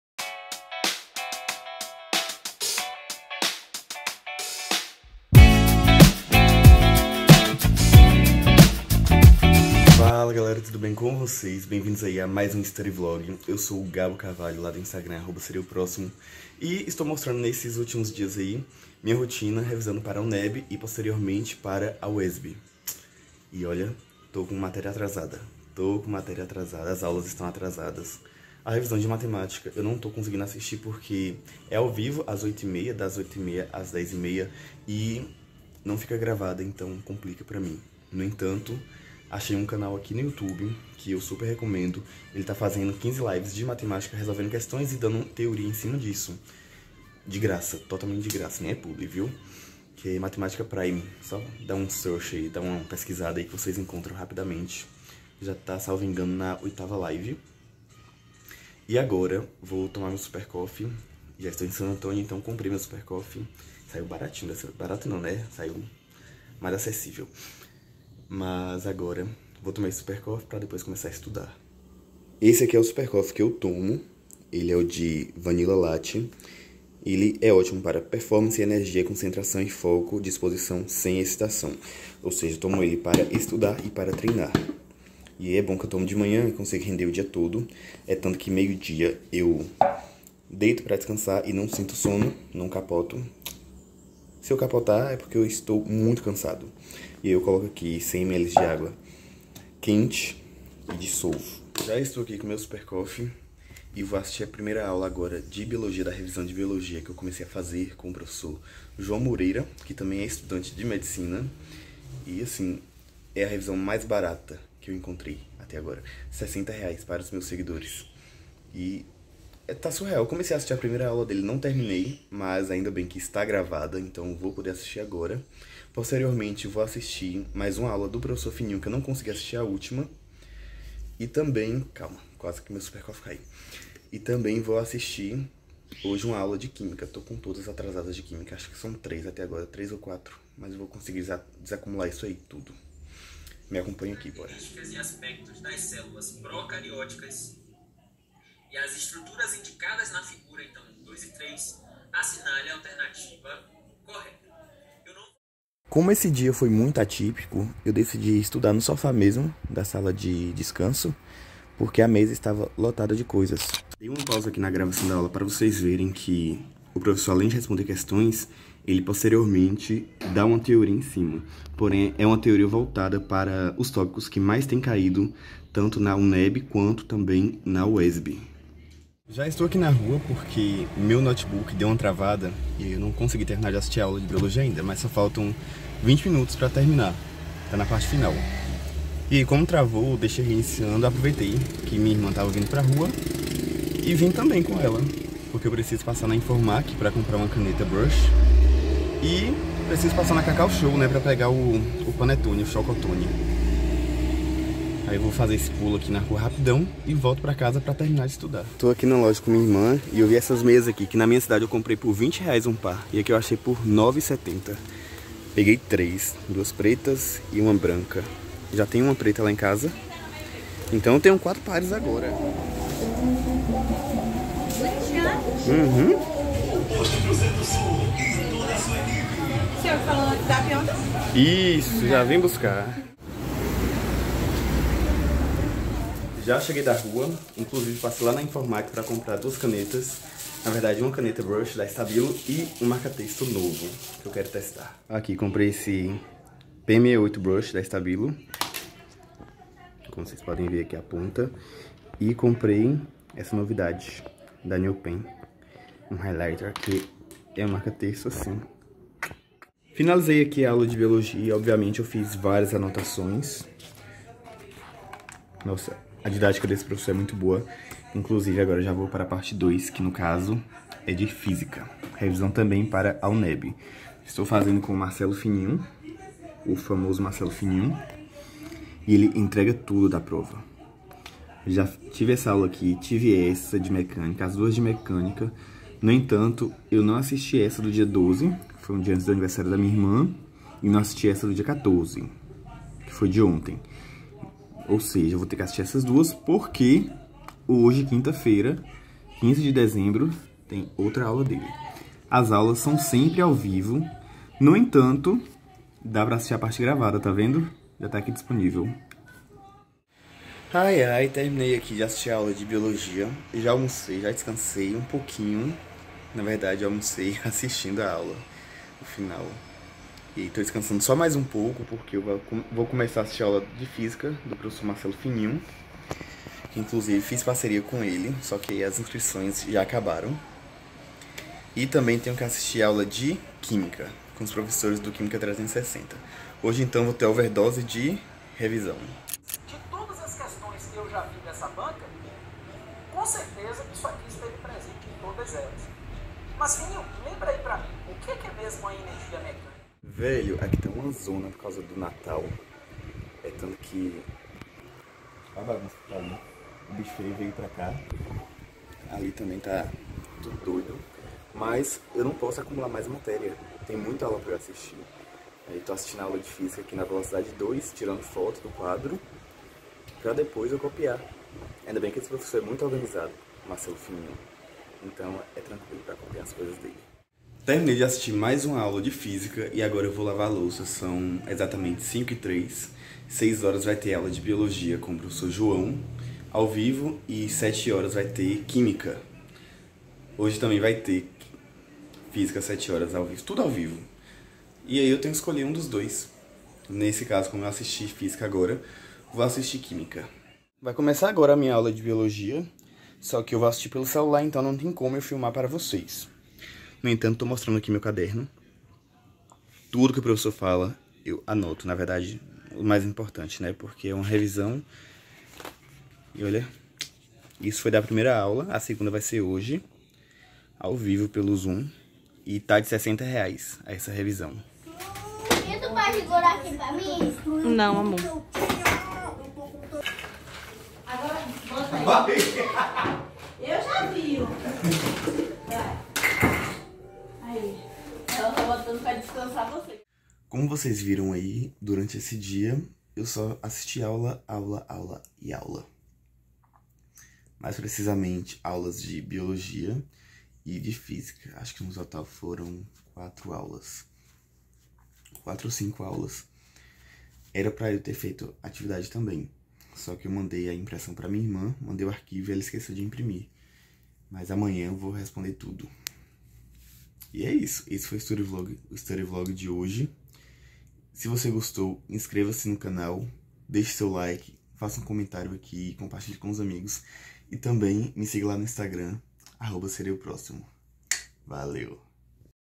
STUDY VLOG #14 | Revisão UNEB + desacumulando as aulas
Esse é mais um study vlog de revisão para o vestibular da UNEB.

Link dos grupos com pessoas que vão fazer os vestibulares da UESB e da UNEB

Isoladas de Revisão

Química: @professorhelan e @professoraugustoheine
Física: @fisicacomfininho
Biologia: @joaomoreira1